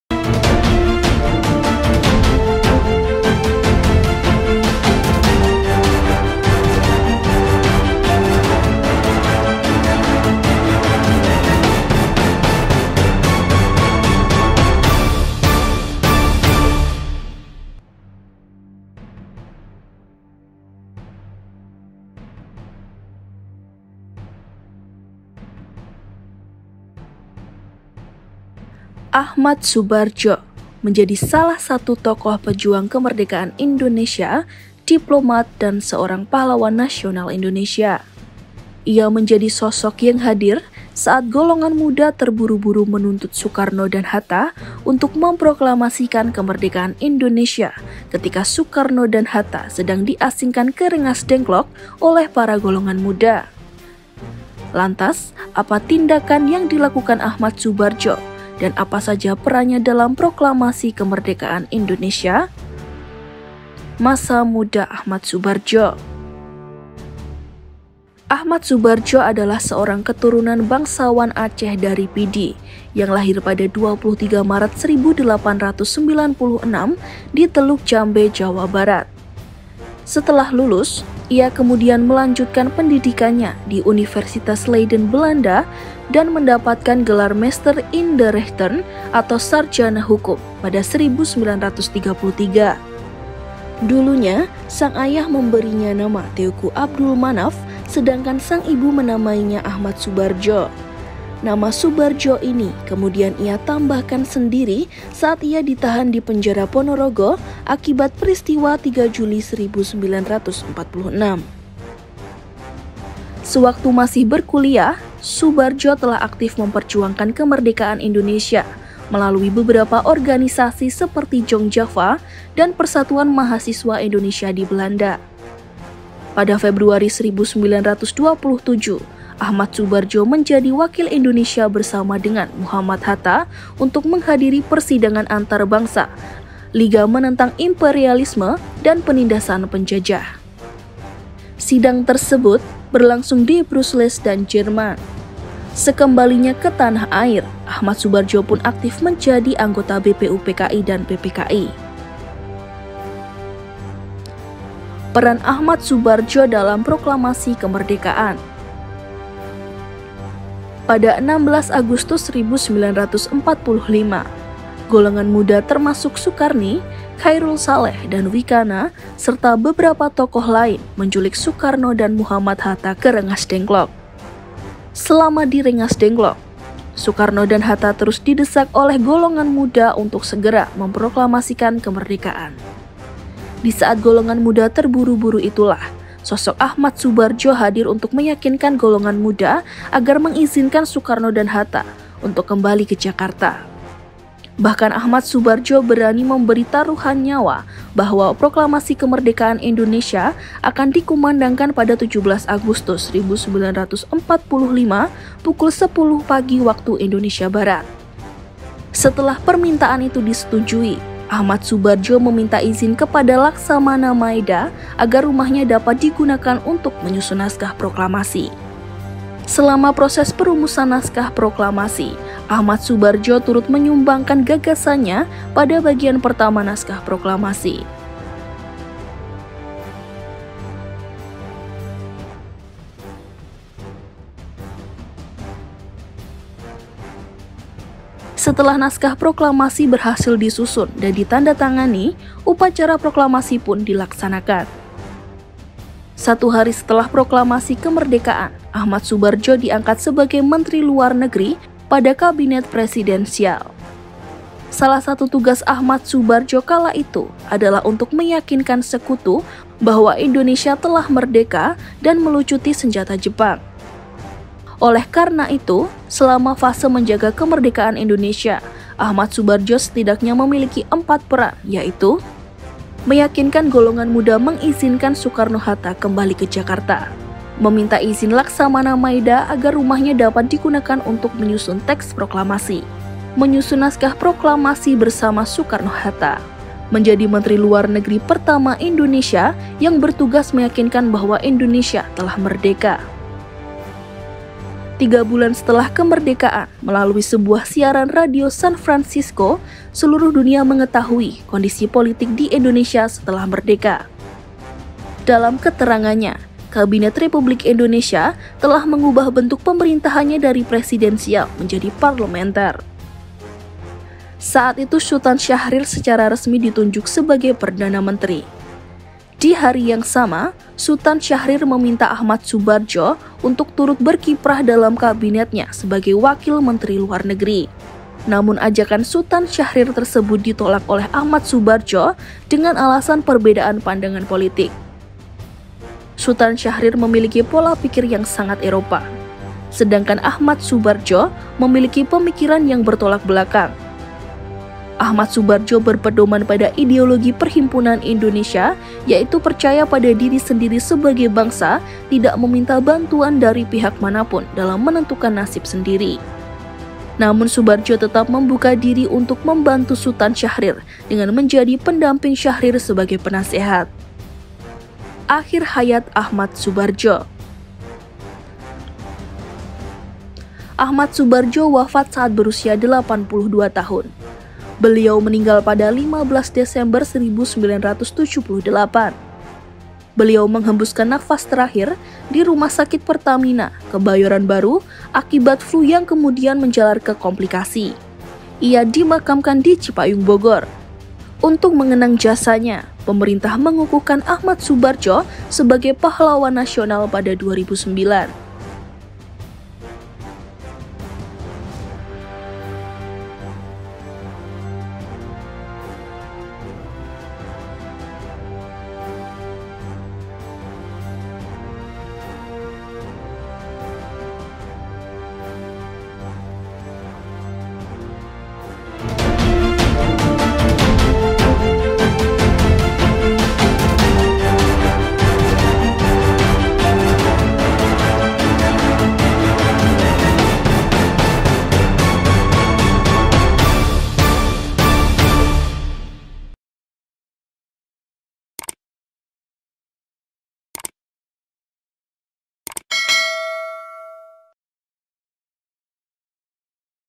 Achmad Soebardja, Sang Penjamin Kemerdekaan Indonesia
Achmad Soebardjo adalah tokoh pejuang kemerdekaan Indonesia, diplomat, dan seorang Pahlawan Nasional Indonesia. Ia menjadi sosok yang hadir saat golongan muda terburu-buru menuntut Soekarno dan Hatta untuk memproklamasikan Kemerdekaan Indonesia ketika Soekarno dan Hatta, sedang diasingkan ke Rengasdengklok oleh para golongan muda.

Lantas, apa tindakan yang dilakukannya dan apa saja peran Achmad Soebardjo dalam proklamasi kemerdekaan Indonesia?

Penulis: Verelladevanka Adryamarthanino
Penulis Naskah: Wiyudha Betha Dinaragis
Narator: Firzha Ananda Putri
Video Editor: Adimas Afif Nugroho
Produser: Firzha Ananda Putri

Sumber Video:
Youtube Arsip Nasional RI